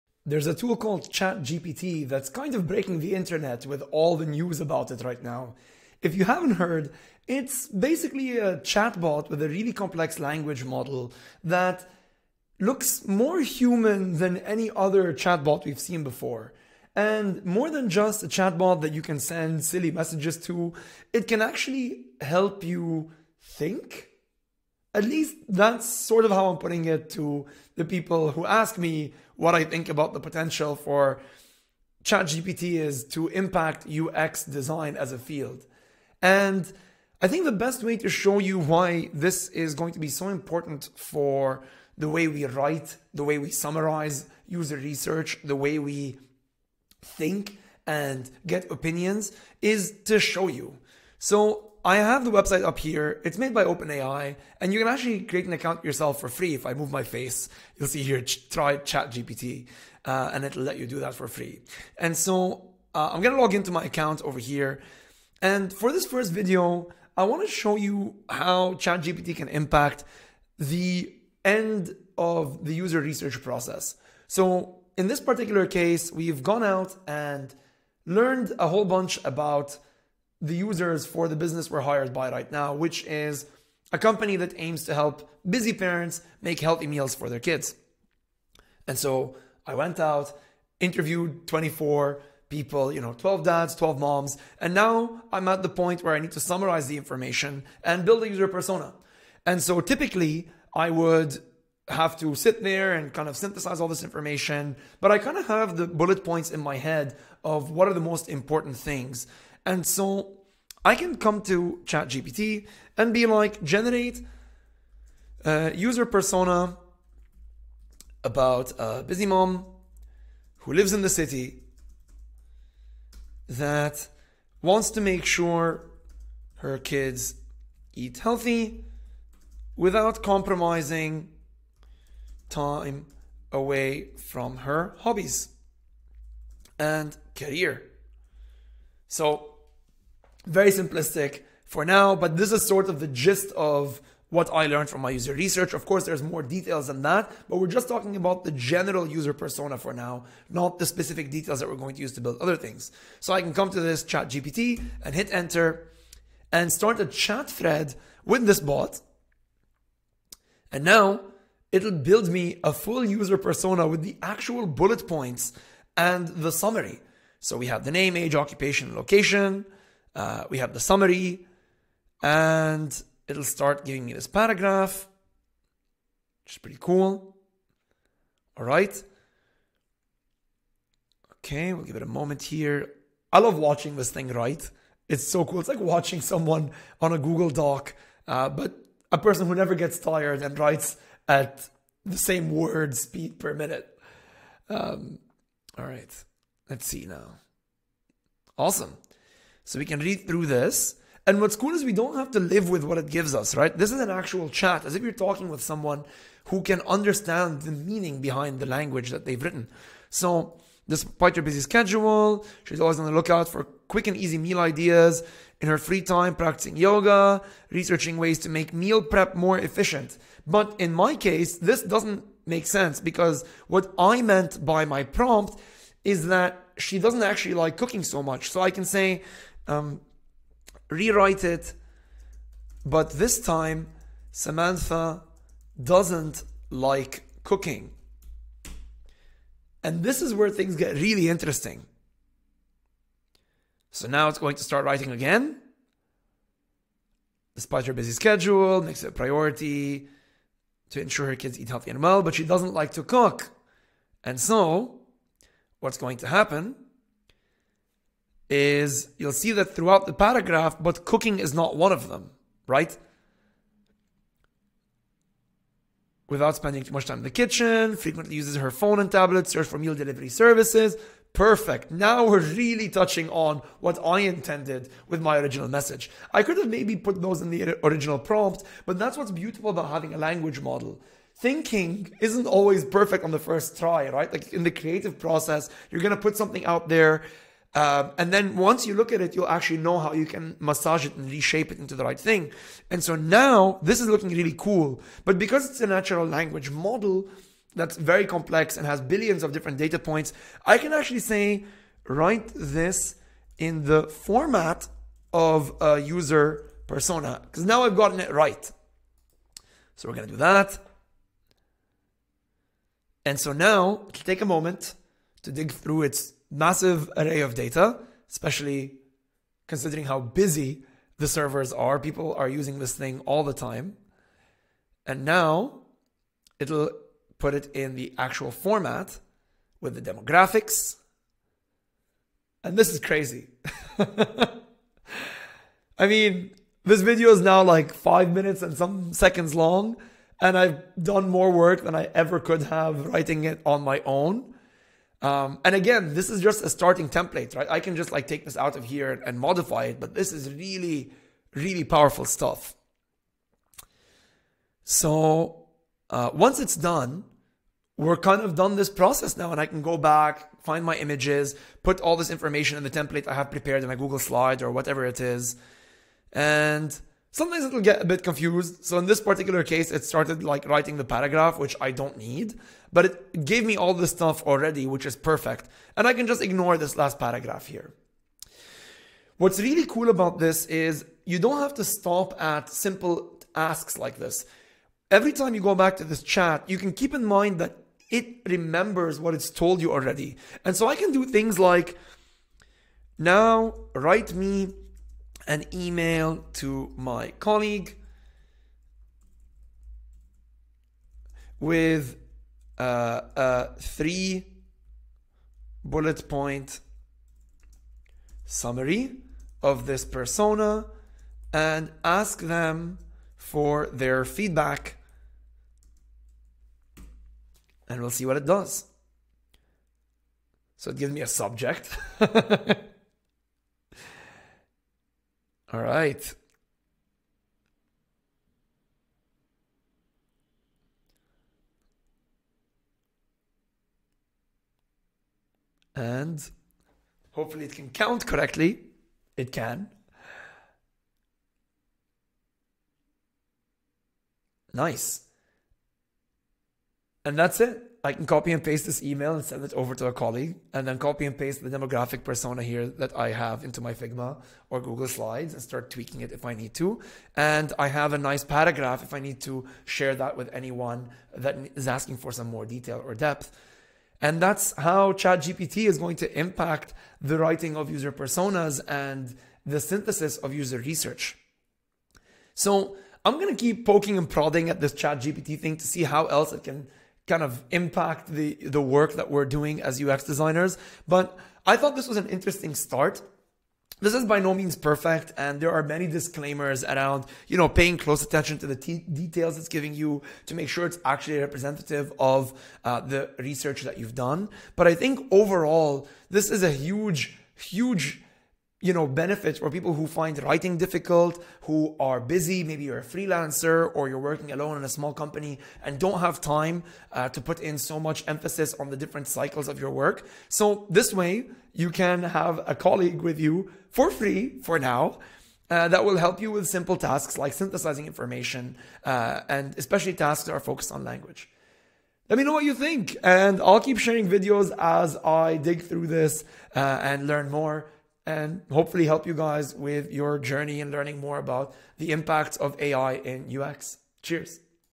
Creating UX Personas with ChatGPT
In this video, we explore how ChatGPT, a state-of-the-art language generation model, can be used to create user personas and summarize user research findings in the field of user experience (UX). We will demonstrate how ChatGPT can generate detailed descriptions of user demographics, behavior, and pain points. This tool makes identifying key takeaways for UX designers a breeze and helps you reflect on what's really important.

AI is here to stay and when used correctly, it can improve the UX research and design process.

0:00 Intro to ChatGPT
1:04 Using ChatGPT for UX Research
1:53 Generating a user persona with a prompt
3:41 Refining the persona with a reply prompt
5:00 Reformatting the persona structure
6:18 Generating an email with a summary of the persona
7:52 Closing remarks